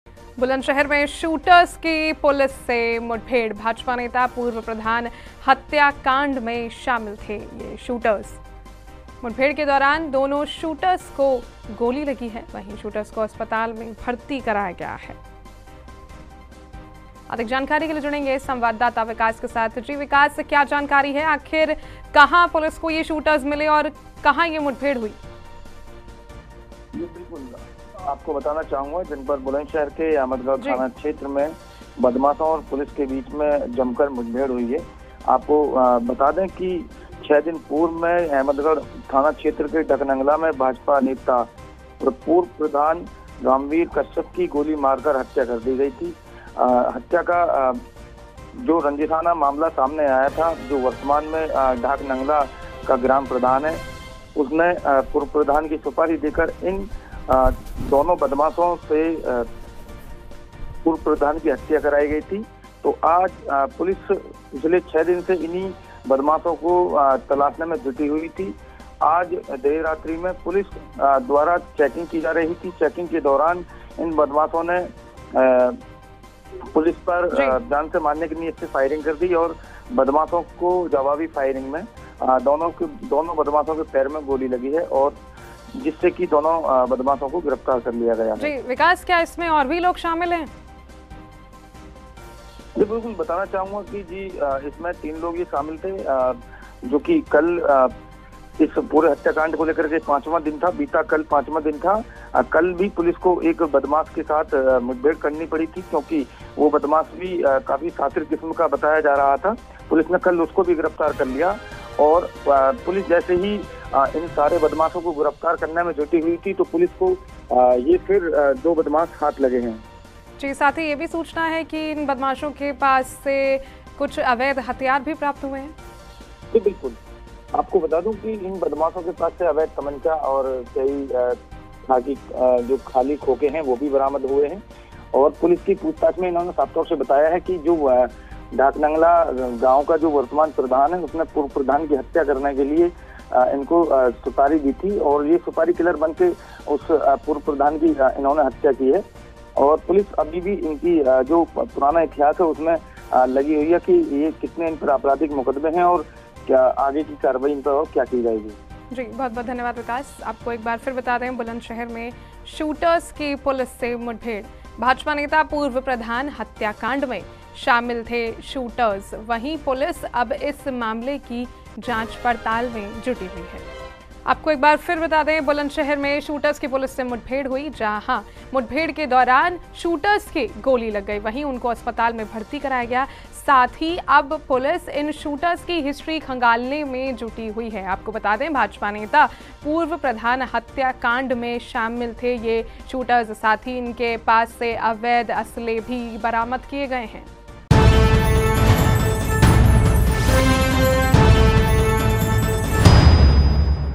Breaing News : बुलंदशहर में शूटर्स की पुलिस से मुठभेड़ |Bulandshahr | Shooters | Encounter |Sea News
बुलंदशहर में शूटर्स की पुलिस से मुठभेड़

भाजपा नेता, पूर्व प्रधान हत्याकांड में शामिल थे शूटर्स

मुठभेड़ के दौरान दोनों शूटर्स को लगी गोली

शूटर्स को अस्पताल में कराया गया भर्ती